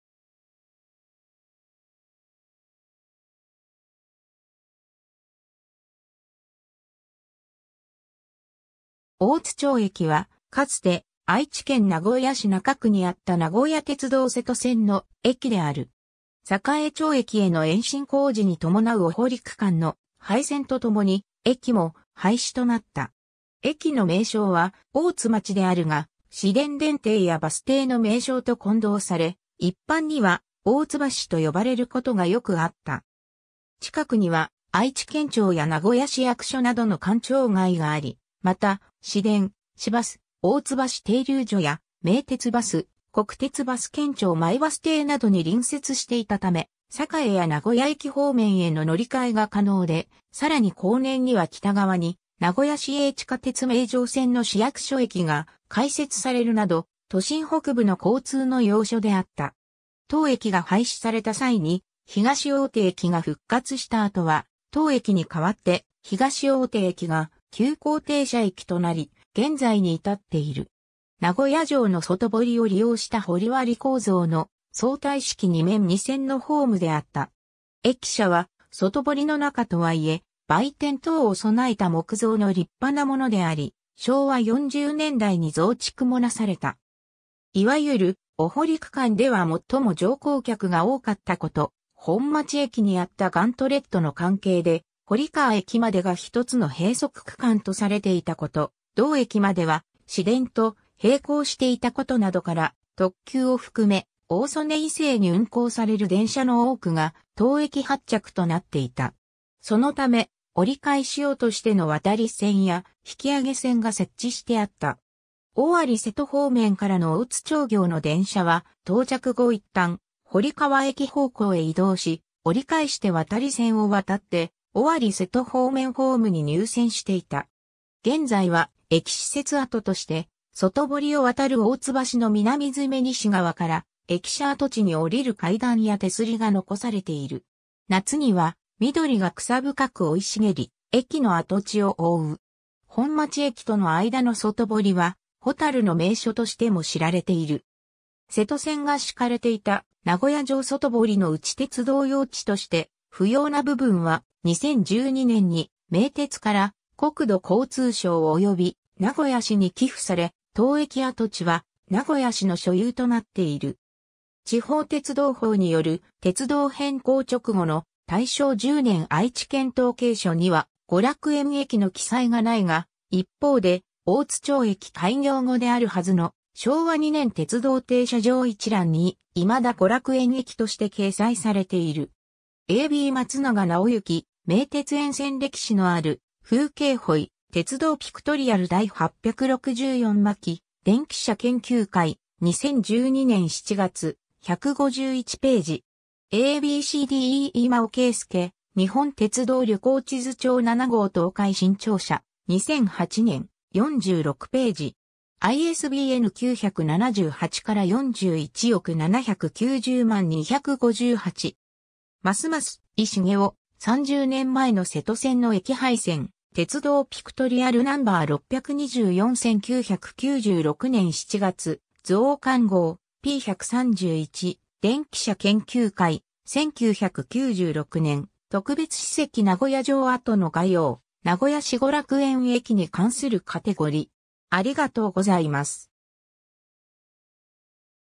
大津町駅 (愛知県)
大津町駅（おおつまちえき）は、かつて愛知県名古屋市中区にあった名古屋鉄道瀬戸線の駅（廃駅）である。
 栄町駅への延伸工事に伴うお濠区間の廃線とともに、駅も廃止となった。
 駅の名称は大津町であるが、市電電停やバス停の名称と混同され、一般には大津橋と呼ばれることがよくあった。
 近くには、愛知県庁や名古屋市役所などの官庁街があり、また、市電・市バス大津橋停留所や名鉄バス・国鉄バス県庁前バス停などに隣接していたため、栄や名古屋駅方面への乗り換えが可能で、さらに後年には北側に名古屋市営地下鉄名城線の市役所駅が開設されるなど、都心北部の交通の要所であった。
 当駅が廃止された際に東大手駅が復活した後は、当駅に代わって東大手駅が急行停車駅となり、現在に至っている。
 名古屋城の外堀を利用した掘割構造の相対式2面2線のホームであった。
駅舎は、外堀の中とはいえ、売店等を備えた木造の立派なものであり、昭和40年代に増築もなされた。
 いわゆる「お濠区間」では最も乗降客が多かったこと、本町駅にあったガントレットの関係で堀川駅までが1つの閉塞区間とされていたこと、同駅までは市電と並行していたことなどから、特急を含め、大曽根以西に運行される電車の多くが当駅発着となっていた。
そのため、折り返し用としての渡り線や引き上げ線が設置してあった。
尾張瀬戸方面からの大津町行の電車は、到着後一旦堀川駅方向へ移動し、折り返して渡り線を渡って、尾張瀬戸方面ホームに入線していた。
 現在は、駅施設跡として、外堀を渡る大津橋の南詰西側から駅舎跡地に降りる階段や手すりが残されている。
夏には緑が草深く生い茂り、駅の跡地を覆う。
本町駅との間の外堀はホタル（ヒメボタル）の名所としても知られている。
 瀬戸線が敷かれていた名古屋城外堀のうち鉄道用地として不要な部分は、2012年に名鉄から国土交通省（国道22号部分）および名古屋市（その他）に寄付され、当駅跡地は名古屋市の所有となっている。
 ^ 地方鉄道法による鉄道変更直後の『大正10年 愛知県統計書』（1921年）には娯楽園駅の記載がないが、一方で大津町駅開業後であるはずの『昭和2年 鉄道停車場一覧』（1927年）に未だ娯楽園駅として掲載されている。
 ^ a b 松永直幸「名鉄沿線 歴史のある風景 補遺」『鉄道ピクトリアル』第864巻、電気車研究会、2012年7月、 151頁。
 ^ a b c d e 今尾恵介（監修）『日本鉄道旅行地図帳 7号 東海』新潮社、2008年、46頁。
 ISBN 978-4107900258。
 ^ 益井茂夫 「30年前の瀬戸線の駅配線」、『鉄道ピクトリアル No.624 1996年7月増刊号』、p.131、電気車研究会、1996年 ^ 特別史跡名古屋城跡の概要 - 名古屋市 娯楽園駅に関するカテゴリ: